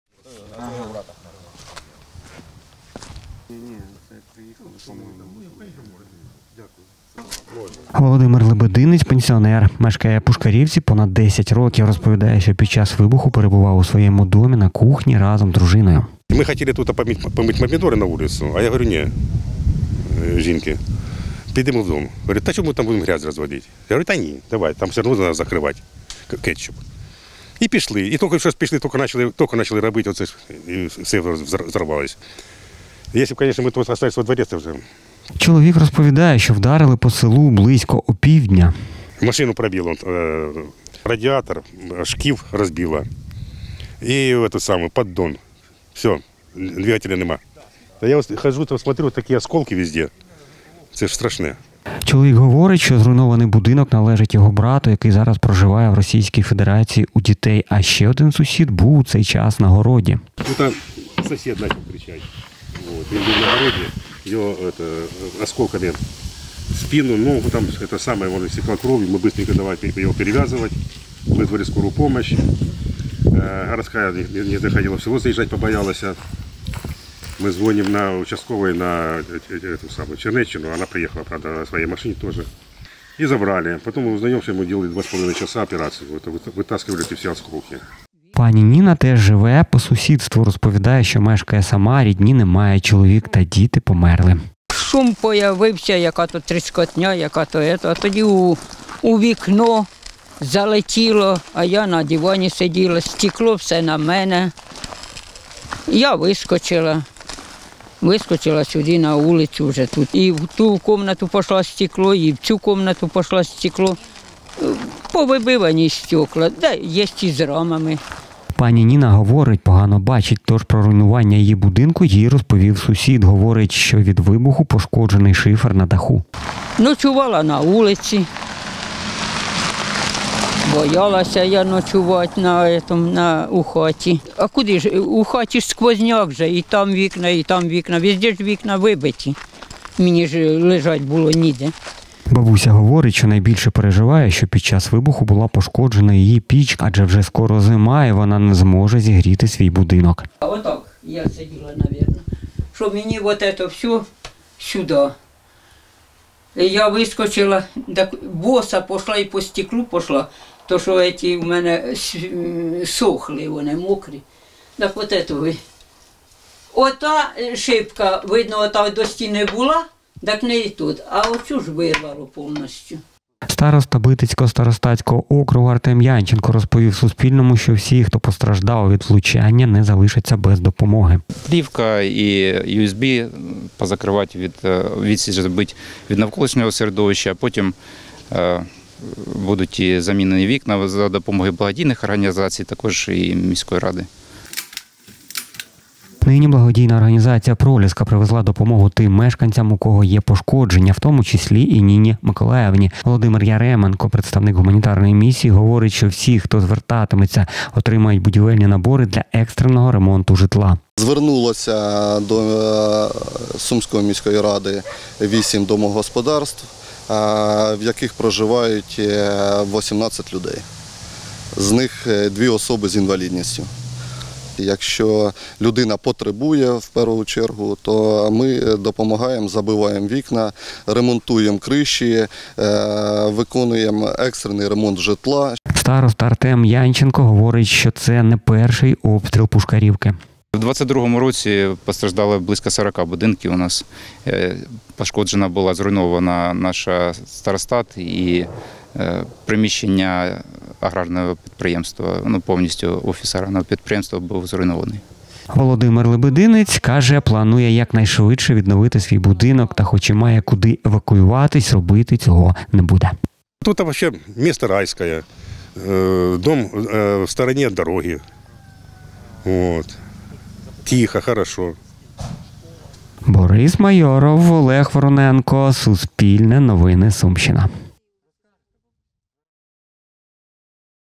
“Я побігла боса по склу”: війська РФ обстріляли село Пушкарівка Битицького старостату
08 серпня близько 12 години у селі Пушкарівка Сумської громади російські військові влучили у житловий будинок. За інформацією ОК “Північ”, яку вони оприлюднили у зведенні на сторінці у Фейсбук,  вдарили по селу керованою авіабомбою. Внаслідок вибуху будівля одного будинку повністю зруйнована, у кількох сусідніх домогосподарствах зруйновані дахи, вибиті вікна. Поранення отримав мешканець Пушкарівки, його відвезли до лікарні. Про це Суспільному розповів староста Битицького старостинського округу Артем Янченко. Кореспонденти Суспільного побували на місці влучання.